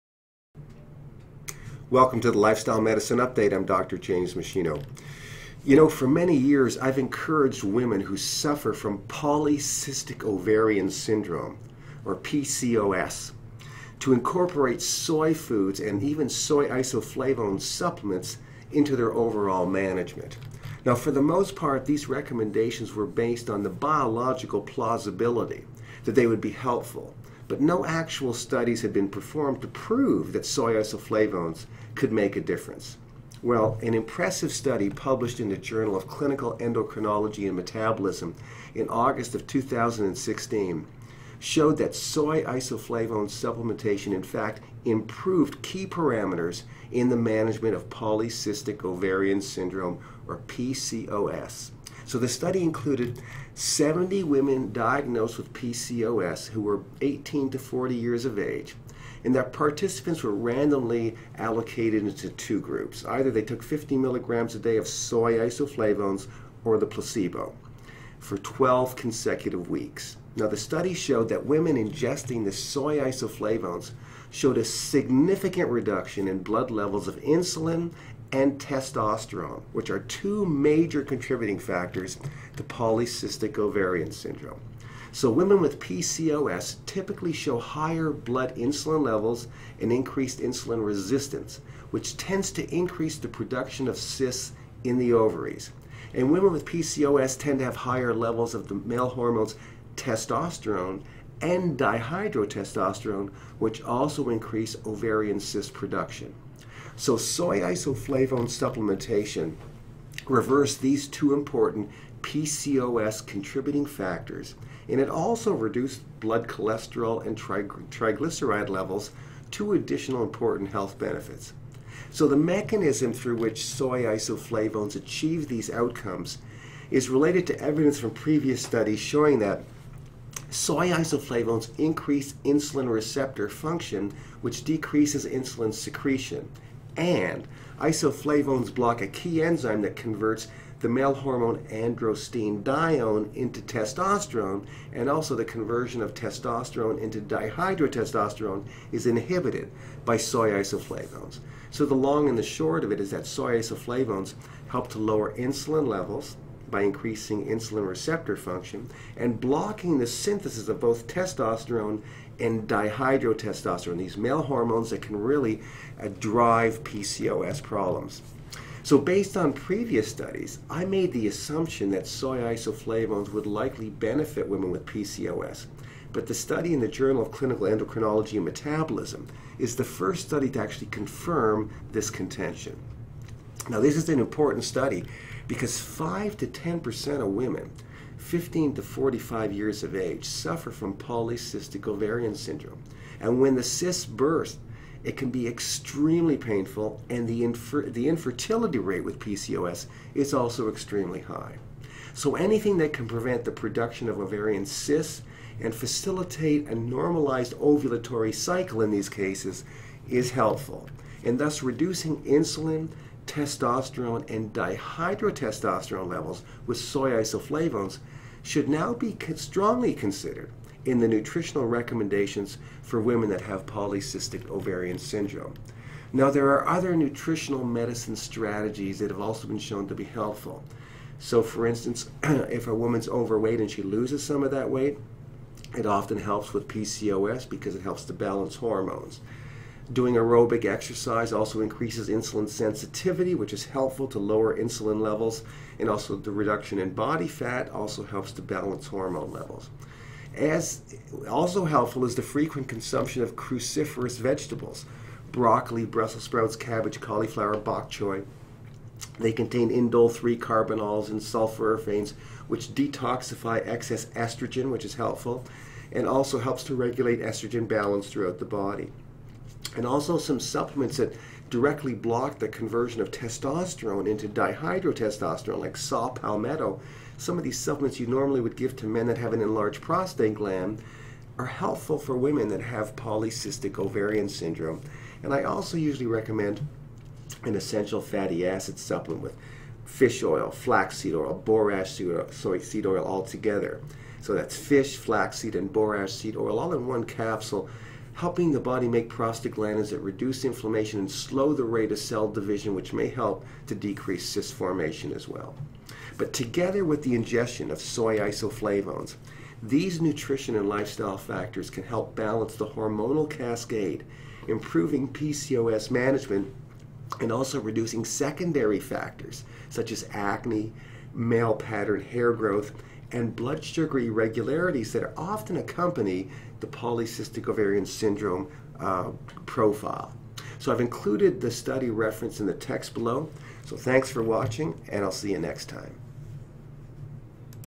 Polycystic Ovarian Syndrome (PCOS) and Soy Isoflavones
💊Recommended Adeeva Supplements💊

💥 Experience the New Way of Aging with the Adëeva Anti-aging 💊Program

Nutrition Medicine Update # 87 - Soy Isoflavones Improve Key Aspect of Polycystic Ovarian Syndrome (PCOS).